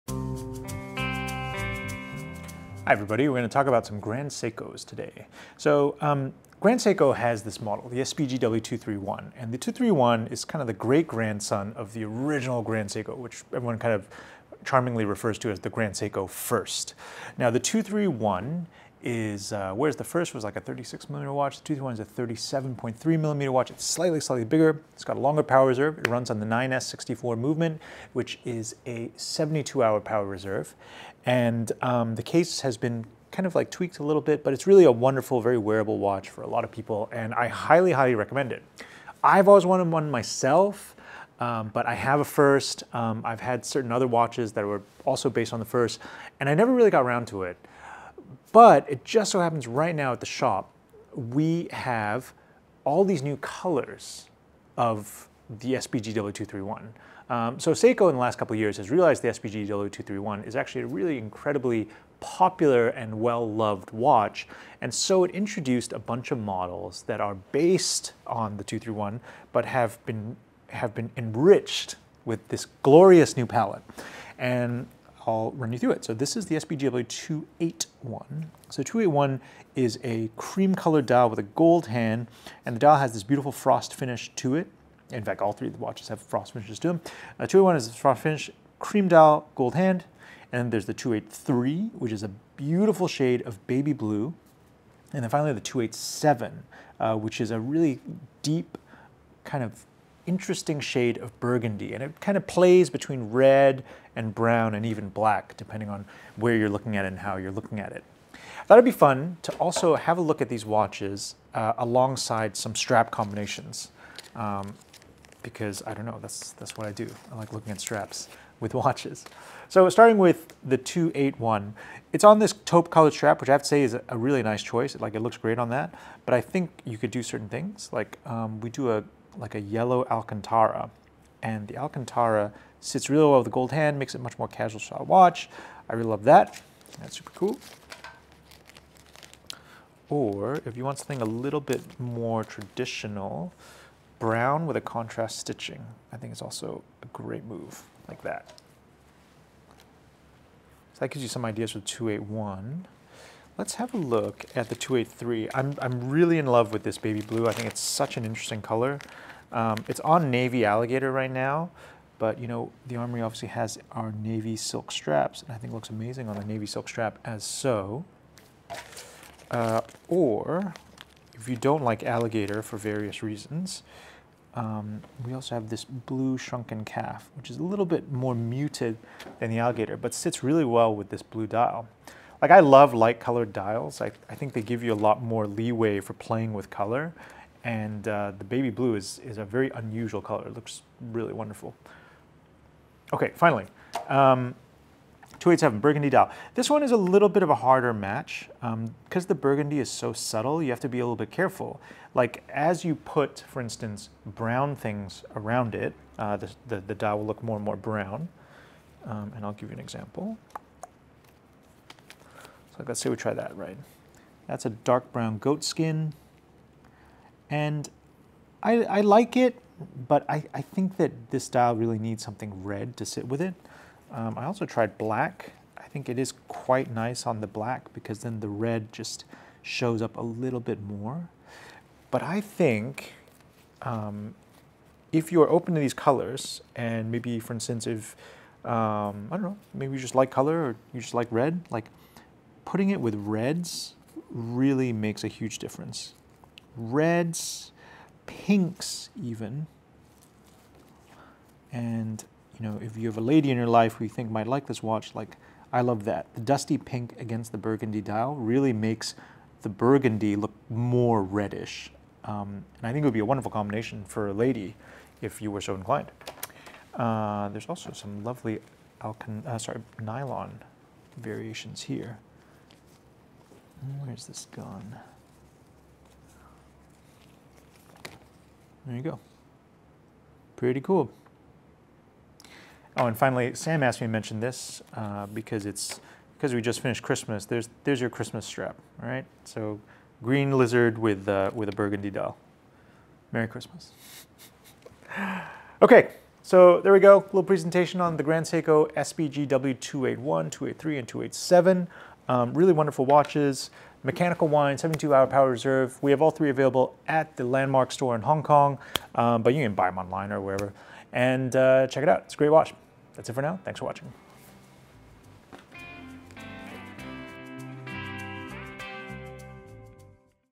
Introducing - An Icon of Grand Seiko in Three New Colours
One of our favourite models in the Grand Seiko range is the SBGW231 because it is a direct descendant of the legendary Grand Seiko First. In this video, we introduce three new colours based on the SBGW231 as well as show some straps to go with them.

Cream - SBGW281
Blue - SBGW283
Burgundy - SBGW287

The Armoury HK is an authorized Grand Seiko dealer. The Armoury HK and NY also sell Jean Rousseau straps ready-made and made-to-order.